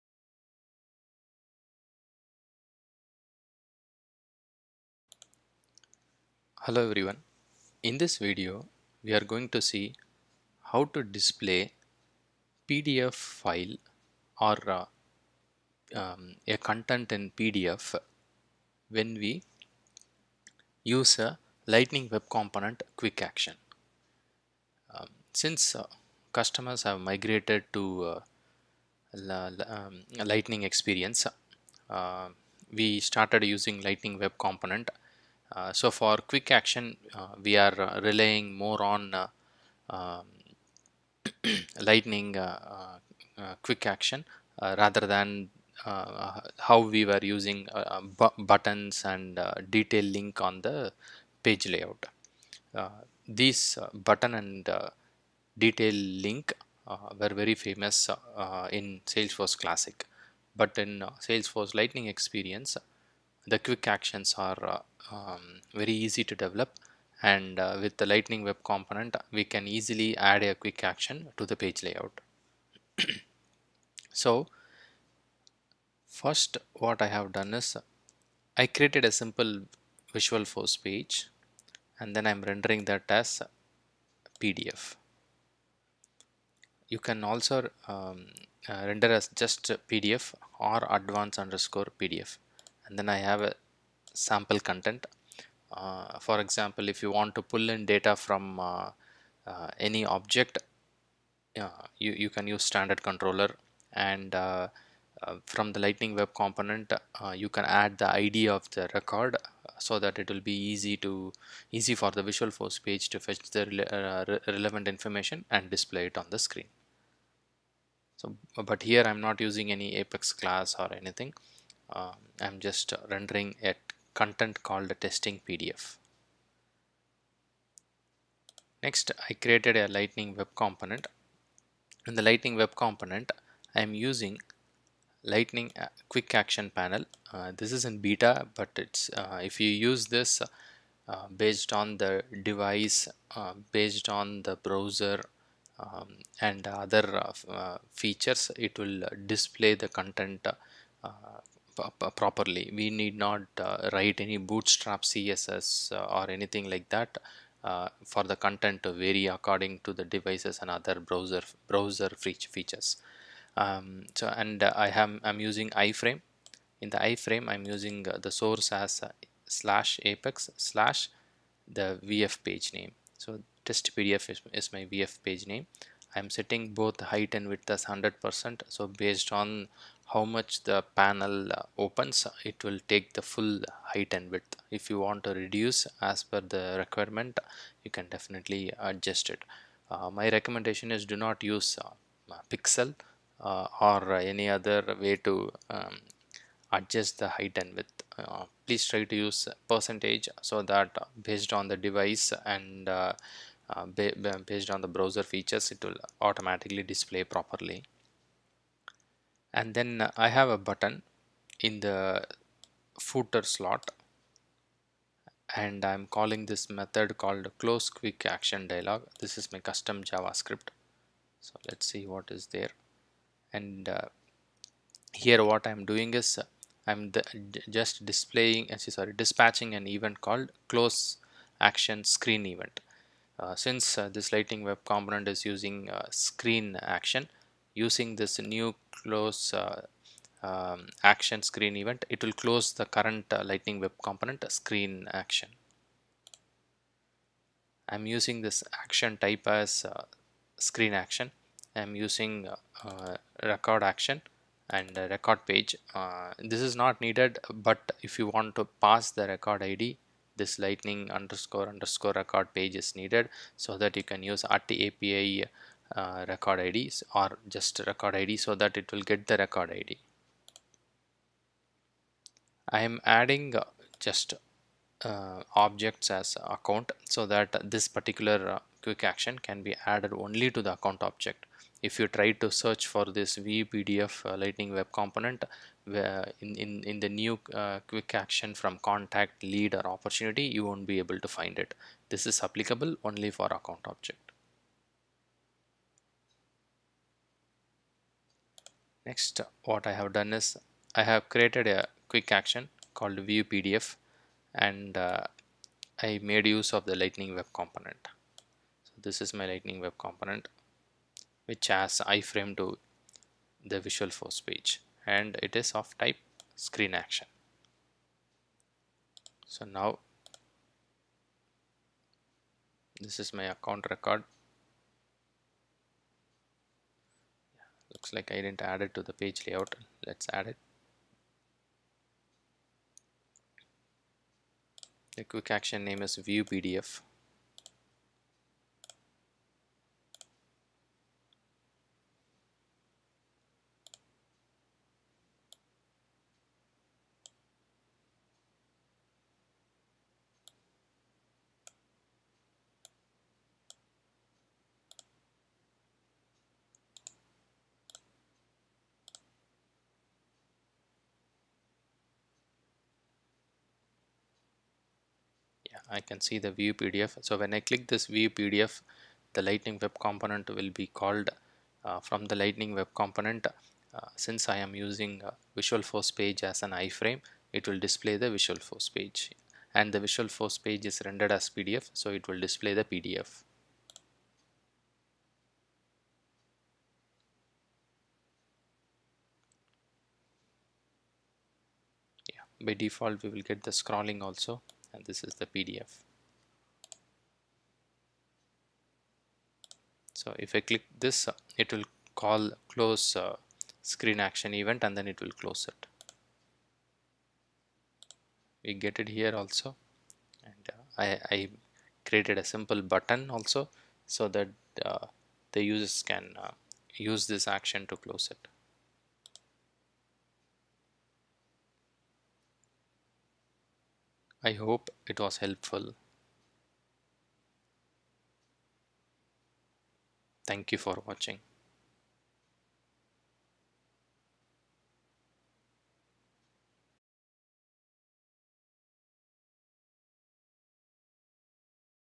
Display PDF from Lightning Web Component Quick Action
Visualforce page with renderAs="advanced_pdf" can be used in Lightning Web Component using iframe tag to render a pdf from Lightning Web Component Quick Action.